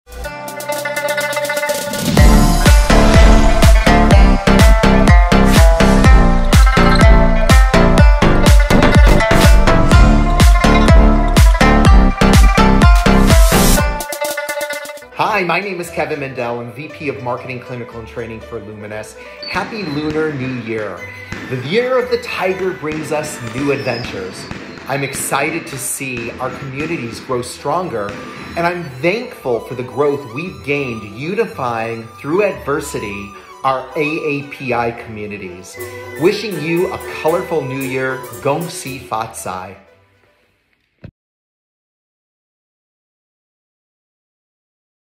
Happy Lunar New Year from Lumenis
At Lumenis, we believe diversity is the standard of beauty. Our advanced technology and branded treatments reflect all six skin types and we're committed to supporting all people.

We are so excited to celebrate the Year of the Tiger with our AAPI community this Lunar New Year! As the VP of Marketing, Clinical, and Training for Lumenis, I am driven to inspire and cultivate inclusive relationships with our partnering providers and continue to celebrate your success in this lunar new year.

We wish you and your family a beautiful and prosperous year!

从不断精进科技造福各种肤色人群，到庆贺不同的文化，科医人坚信美是多元的！疫情没有阻挡我们的成长。这个新春佳节令我们之间的连结更加紧密。
祝愿所有人虎年大吉！吉祥如意！意气风发！发，发，发量感人！（笑）